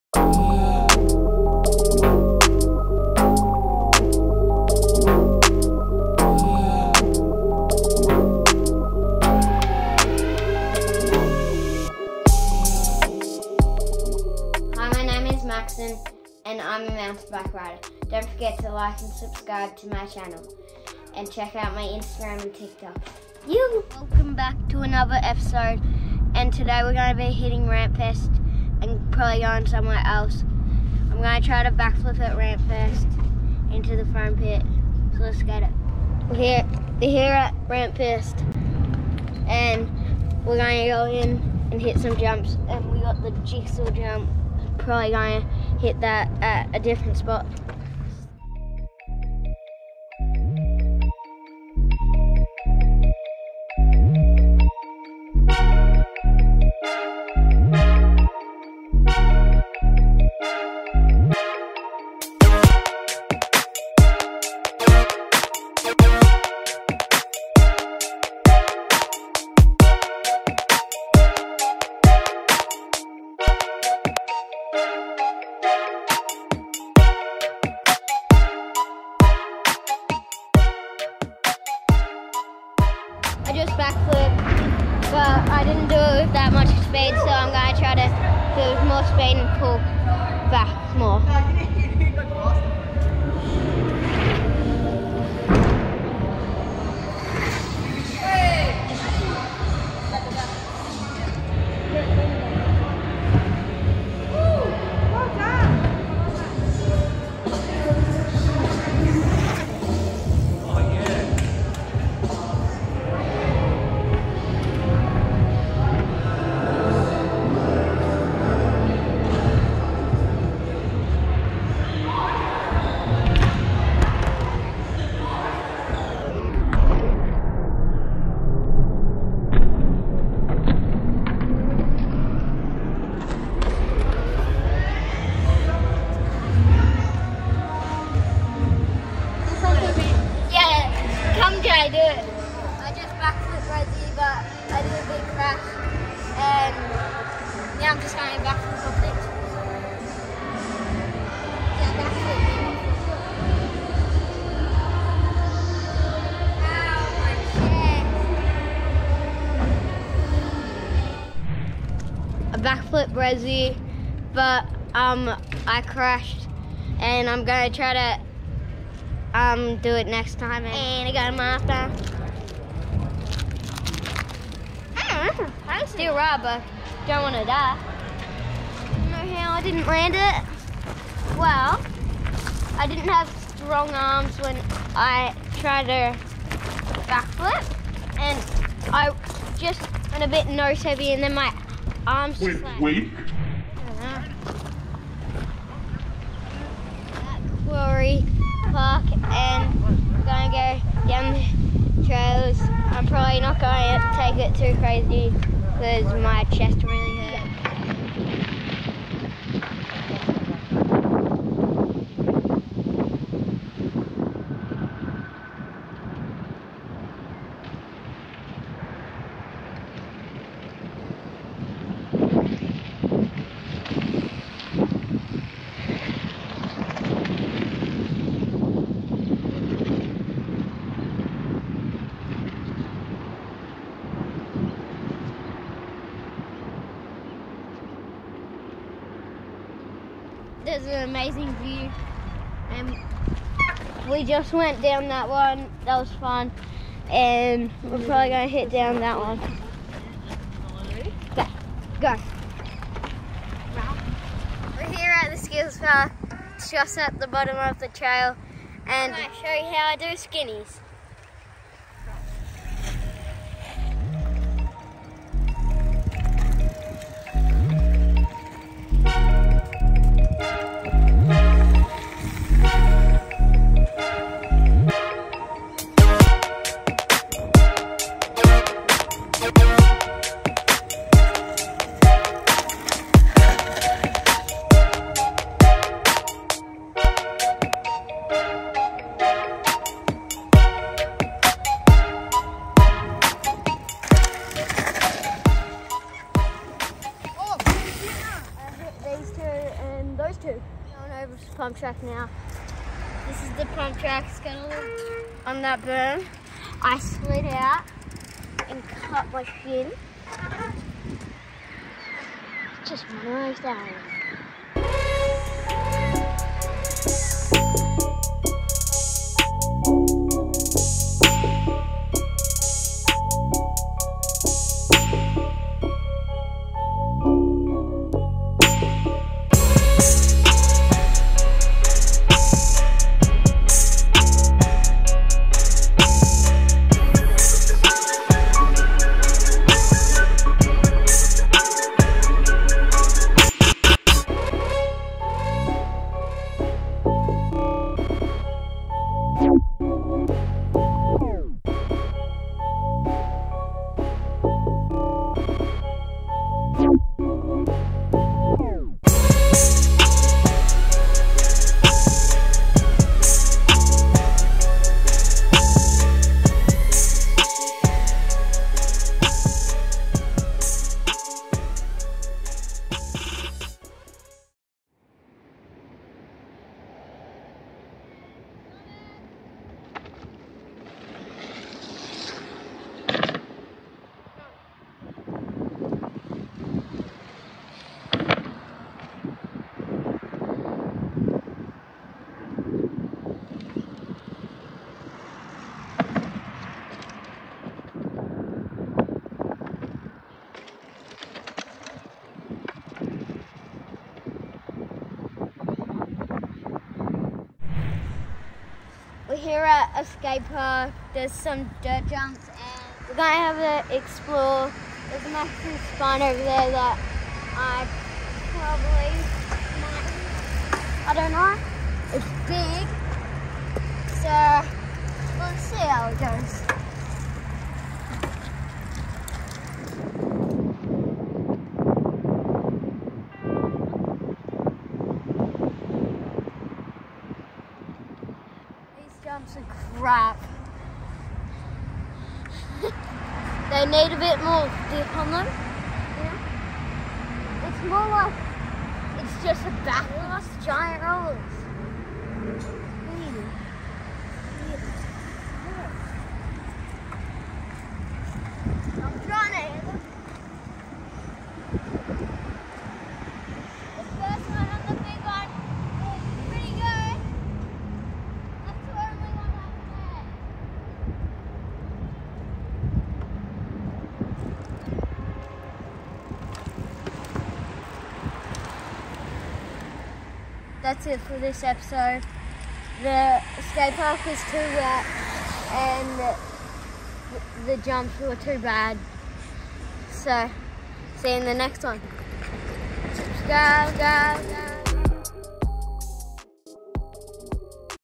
RAMPFEST first backflip to resi
In this episode I go to rampfest a indoor skatepark and do a backflip into foam pit and I do my first backflip onto the resi but I crash. We also go to some tails and quarry park and hit the pump track.